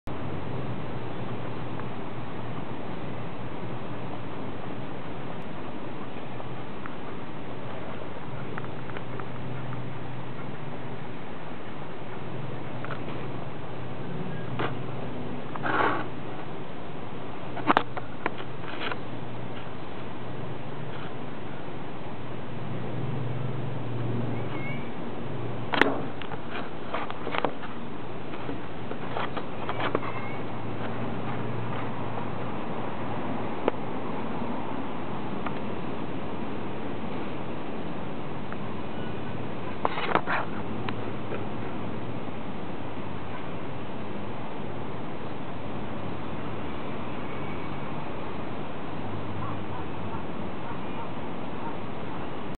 動物園が休みだったから外から望遠でリスザルを撮ってみた
自然動物園　 江戸川区